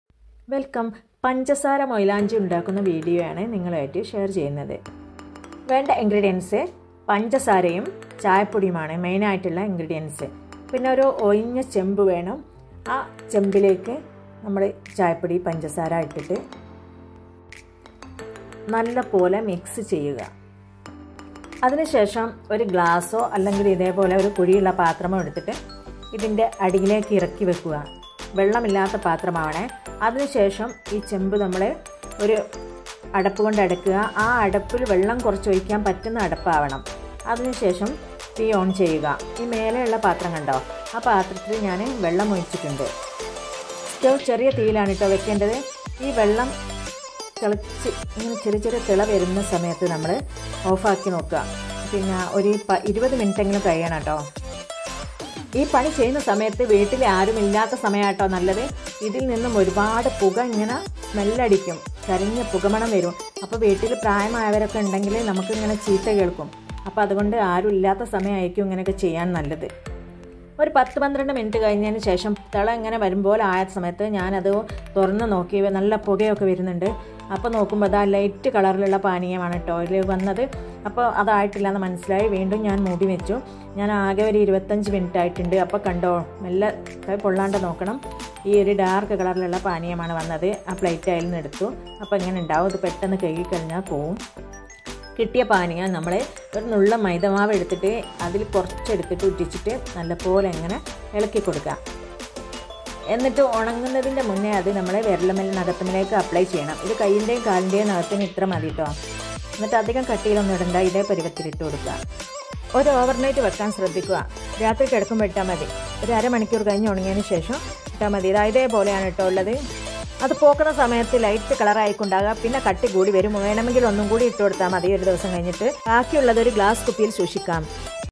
നഖങ്ങൾ ചുവപ്പിക്കാൻ മൈലാഞ്ചി സൂക്ഷിച്ചും വെക്കാം സമയം കളയാതെ ഉണ്ടാക്കാം Mehandi making for Tea
Shukar mehandi
Make a video
Panchasara
Tea powder only ingredients
Veri good red colour for nails